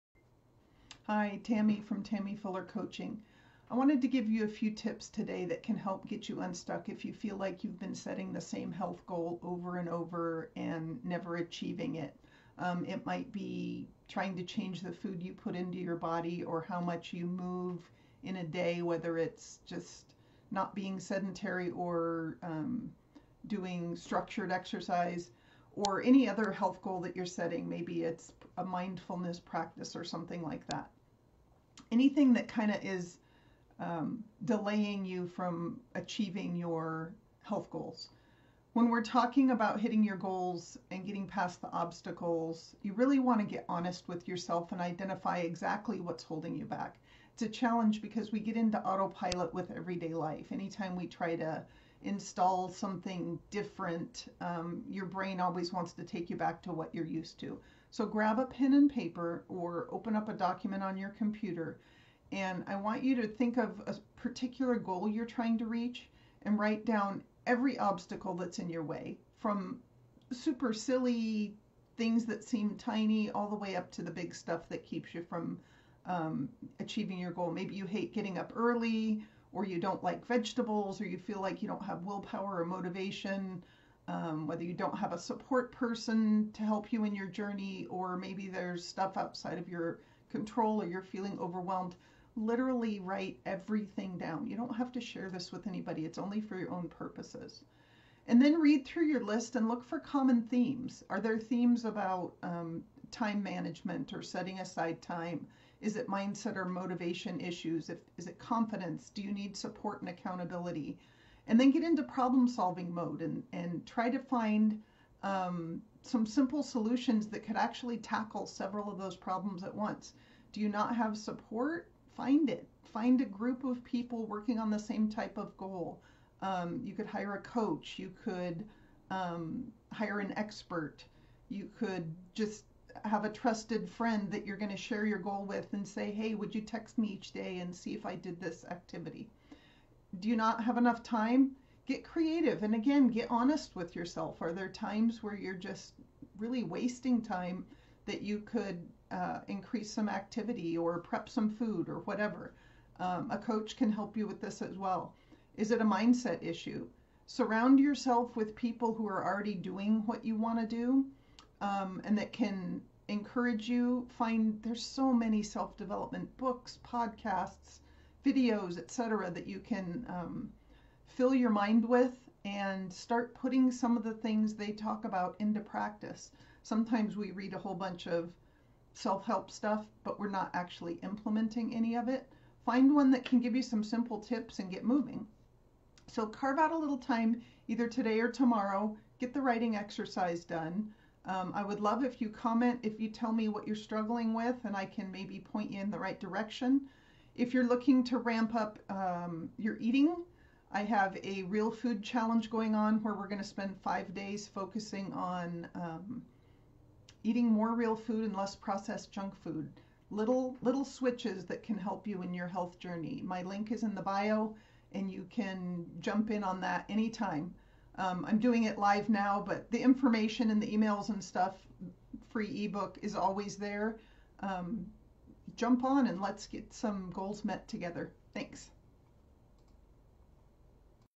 Are you stuck when it comes to accomplishing a health goal?
Are you stuck when it comes to accomplishing a health goal? Take a few minutes today to brainstorm your obstacles and solutions.